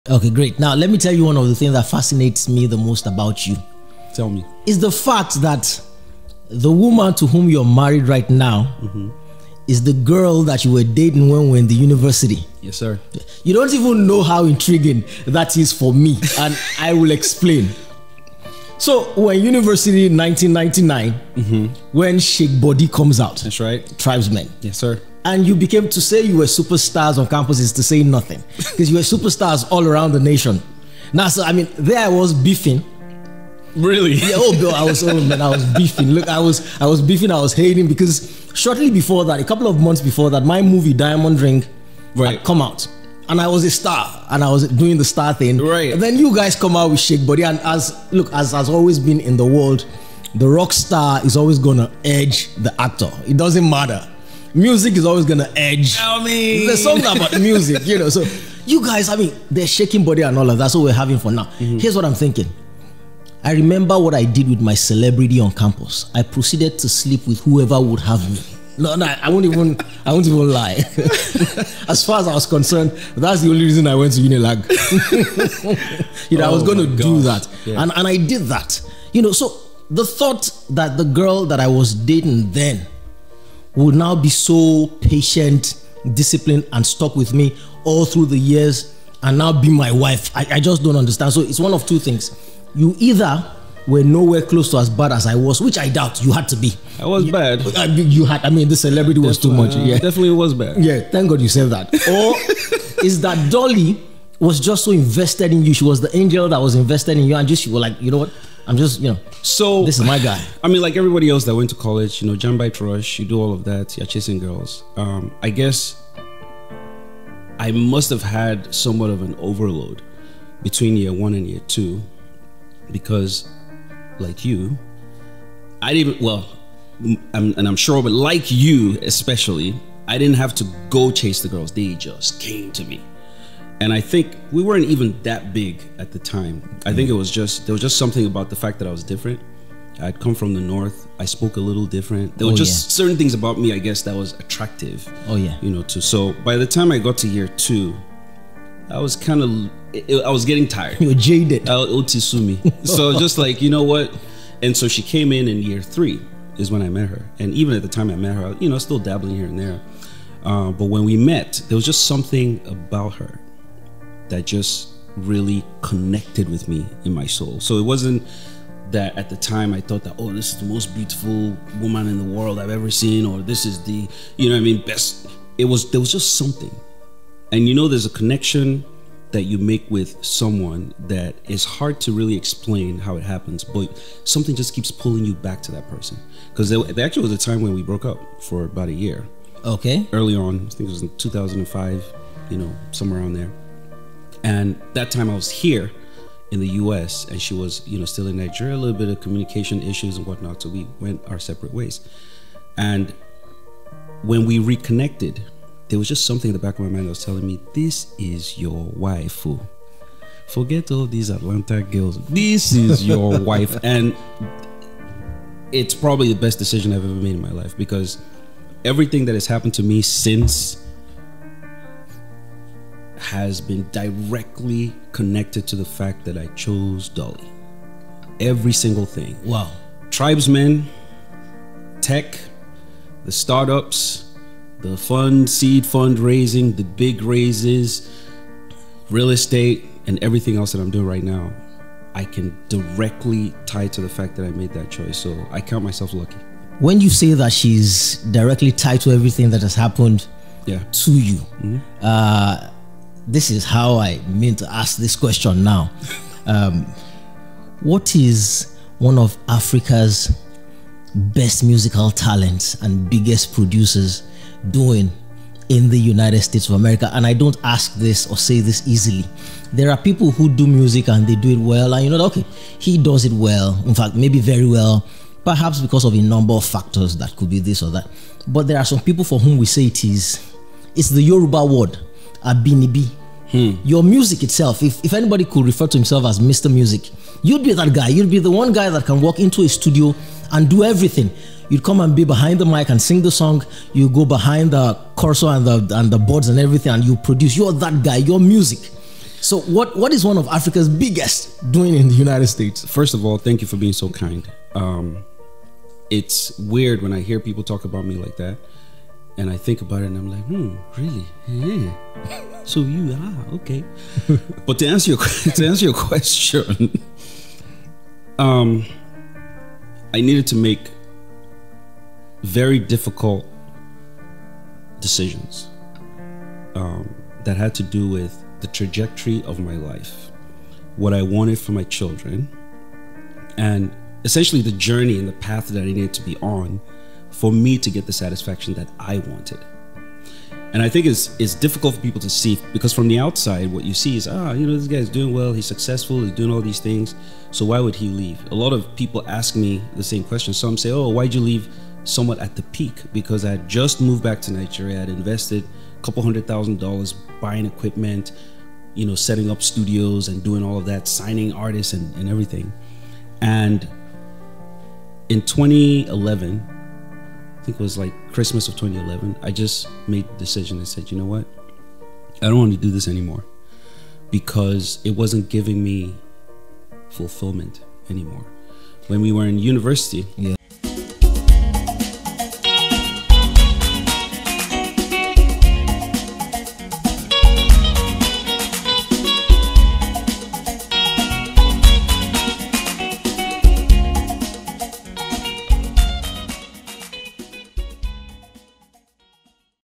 THIS IS YOUR WIFE O! FORGET ALL THESE ATLANTA GIRLS.. ELDEE THA DON
@eldeethedon has been with the same woman @realtordolly for almost 25 years now! And he’s just 46 years old. That means he’s been committed since he was circa 21 years old. That is saying something, isn’t it?